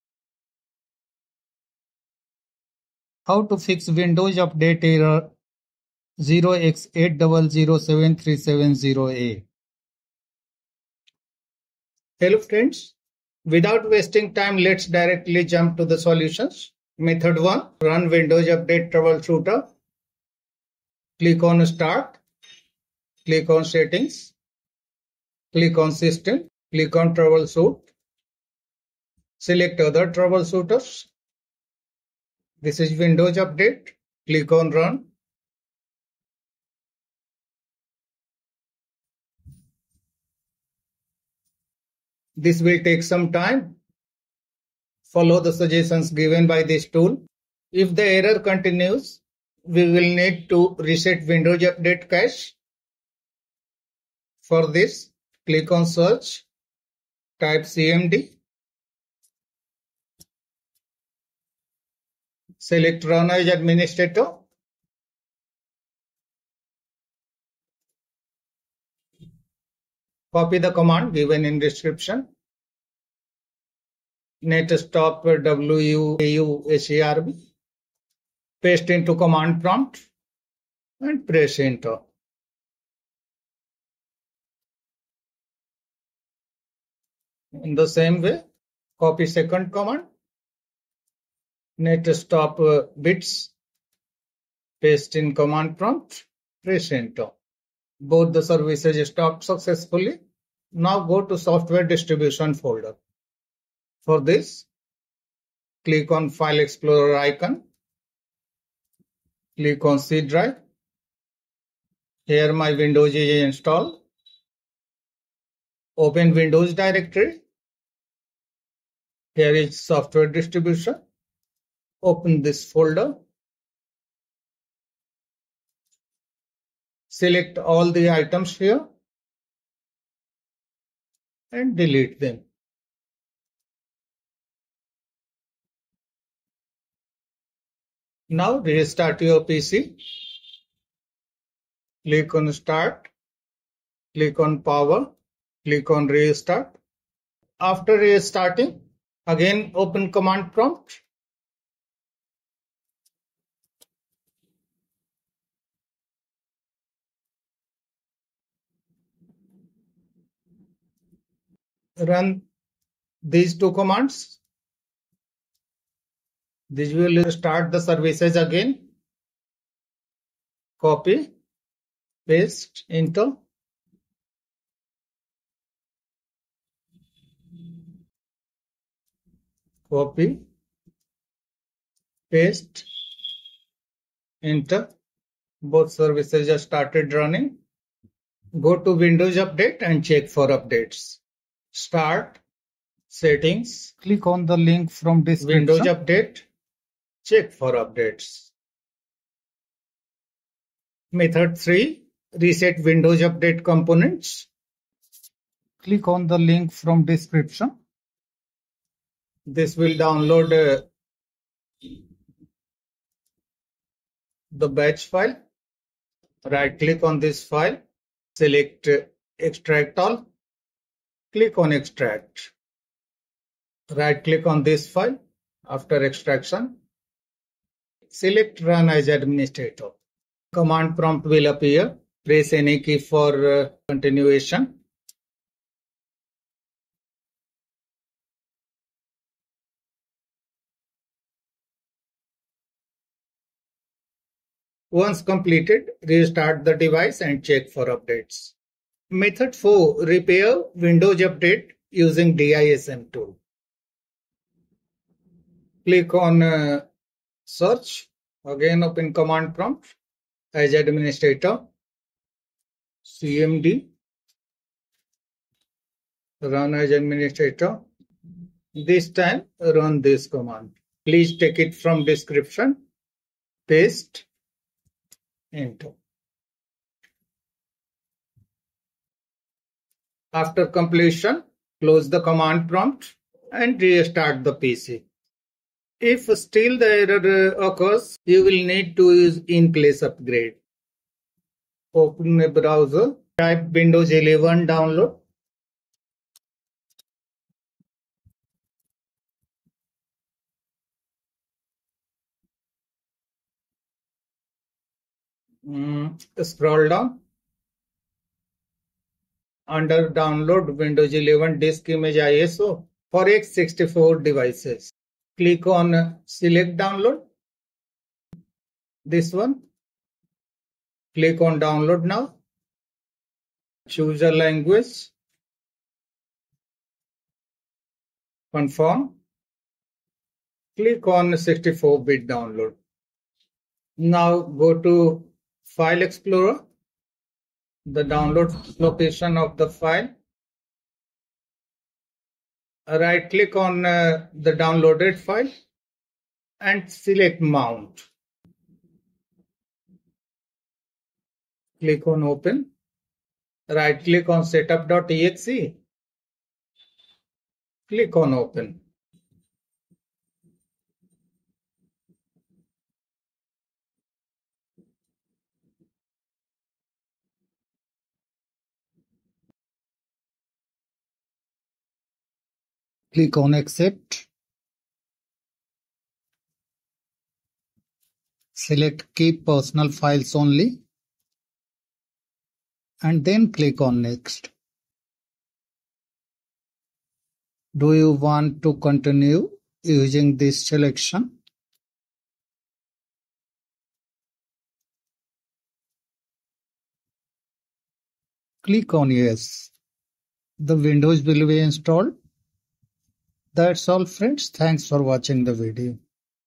How to fix Windows update install error 0x8007370a
This video will guide how to fix Windows update install error 0x8007370a.

Follow the steps:

Method-1: Run Windows Update troubleshooter

• Click on Start.
• Select Settings.
• Click on System.
• Click on Troubleshoot.
• Select Other troubleshooters.
• Follow the suggestions given by the tool after the process is finished.

Method-2:  Delete Windows Update Cache

• Click on Search.
• Type cmd.
• Select Run as administrator.

• Click on File Explorer icon.
• Click on C drive.
• Double click on Windows directory.
• Find SoftwareDistribution folder and open it.
• Select all the files and folders.
• Delete all.
• Close all folder, files, and docs, and restart your PC.
• Again, click on Search.
• Type cmd.

Go to Start - Settings – Windows update – check for updates.

Method-3: Reset Windows Update Components

To reset Windows Update Components, you need to run a great number of commands. This time taken and there are chances of mistakes when running it.

• Right click on this WUReset 2.0.zip and select Extract all.
• Right click on the file and select Run as administrator.
• Once complete restart the device and check for updates.

Method-4: Repair Windows Update using DISM Tool

You will then be required to run the following command:

DISM.exe /Online /Cleanup-Image /RestoreHealth /Source:C:\RepairSource\Windows /LimitAccess

Method-5: Use In-Place upgrade

If still the error occurs, the use In-Place upgrade